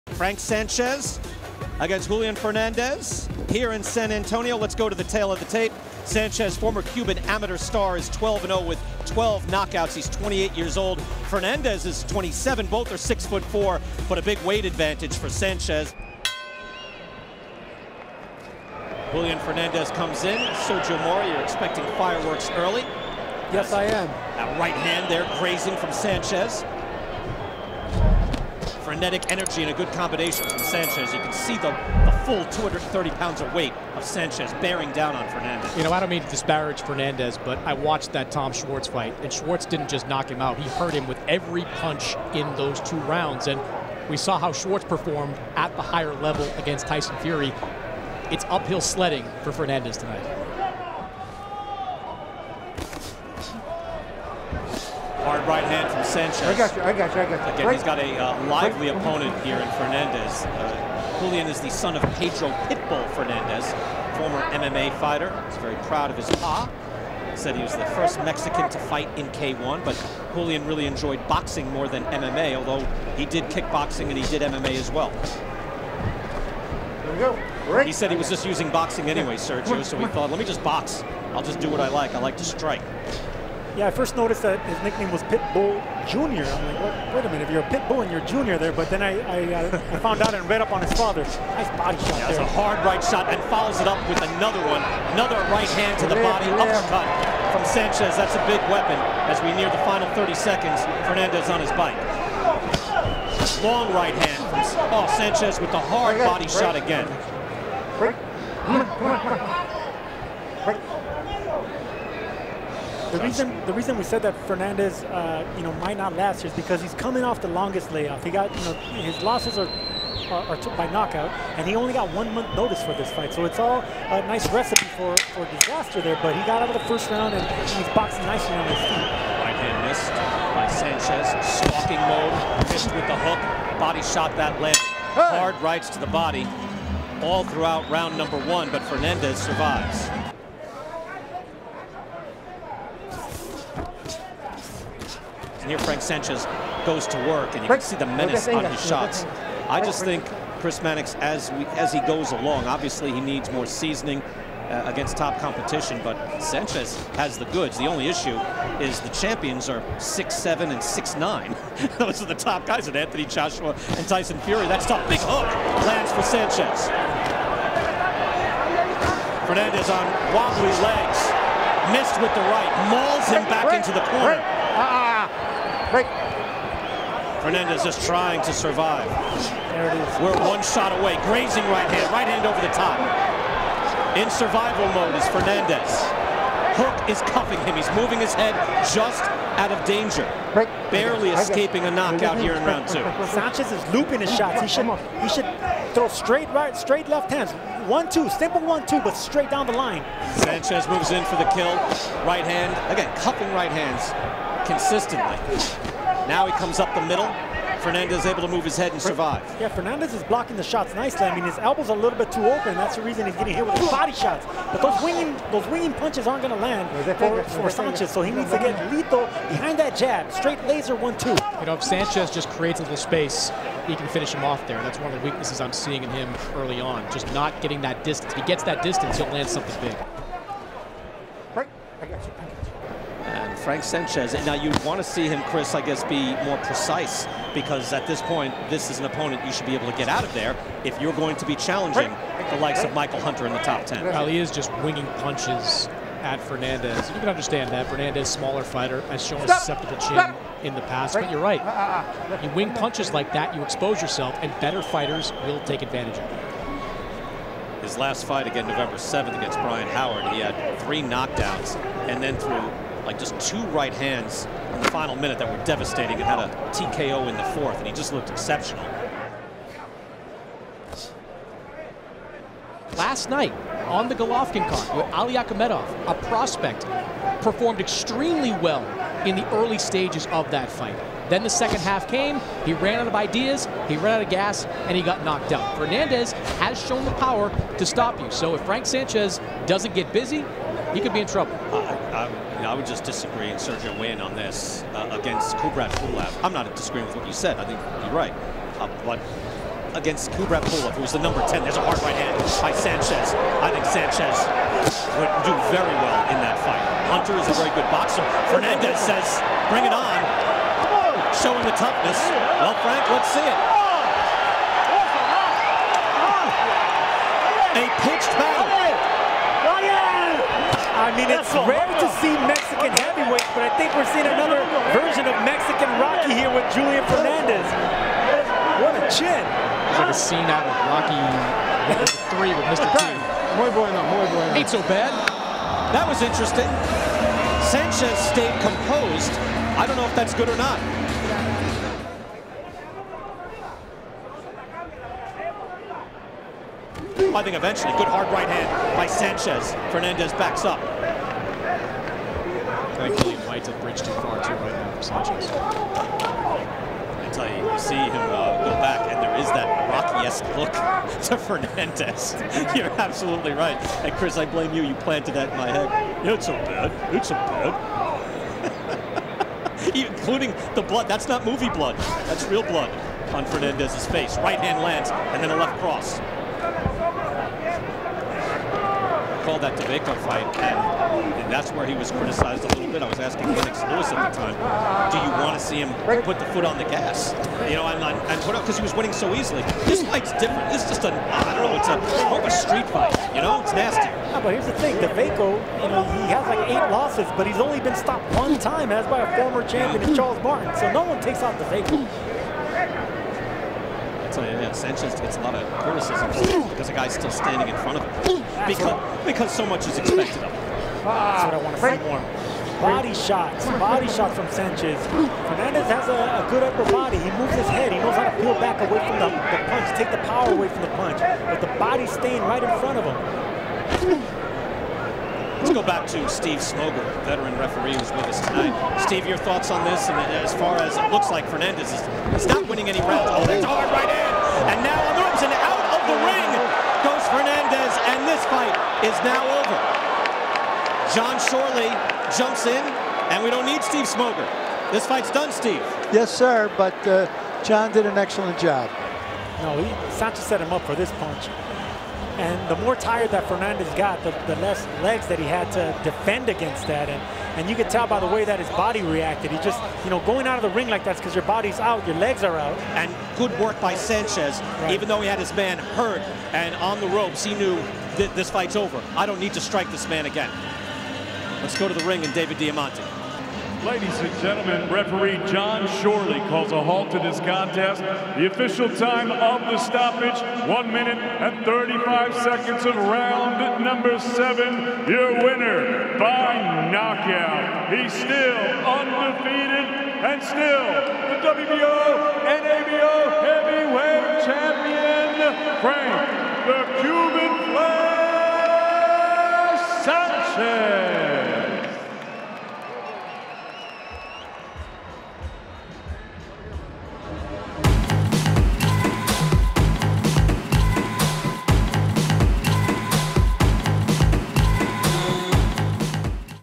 HIGHLIGHTS | Frank Sanchez vs. Julian Fernandez
December 19, 2020 -- Frank Sanchez vs. Julian Fernandez from the Alamodome in San Antonio, Texas.